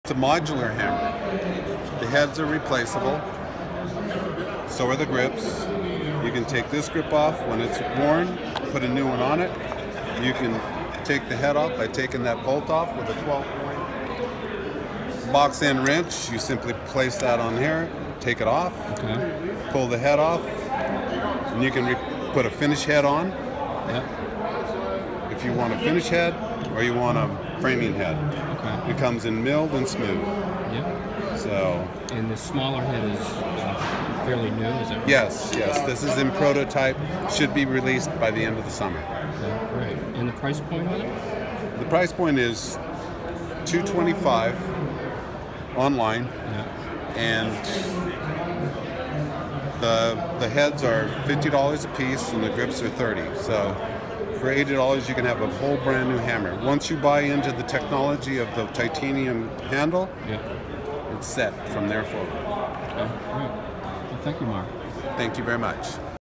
The M-1 Modular Hammer
Mark Martinez of Martinez Tool Company demonstrates the M-1 hammer with interchangeable heads and grips.